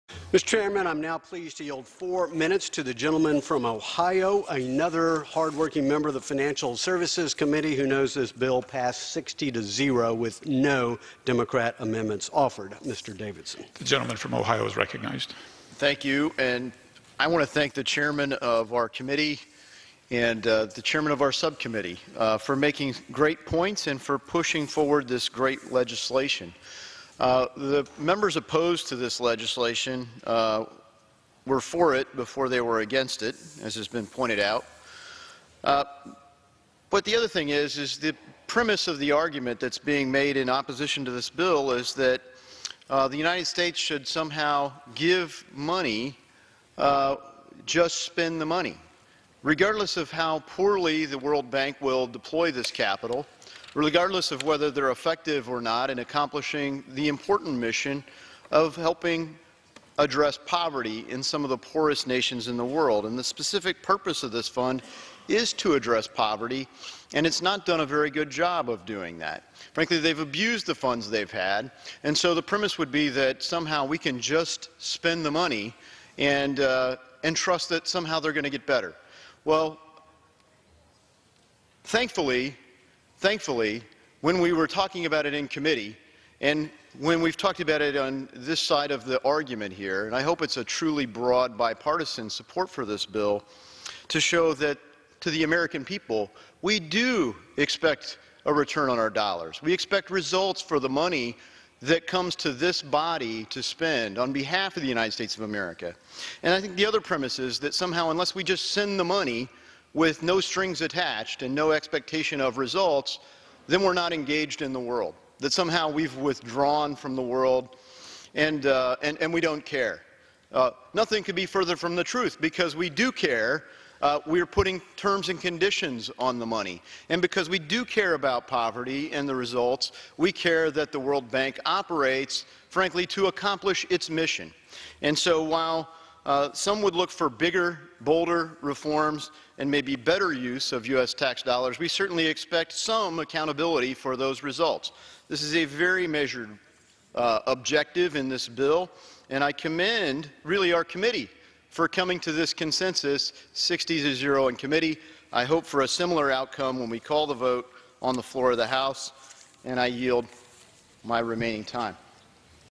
More accountability at the World Bank
Congressman Davidson spoke in support of H.R. 3326, legislation that would put terms and conditions in place of World Bank lending activities that prioritize delivering real results over pushing money out the door.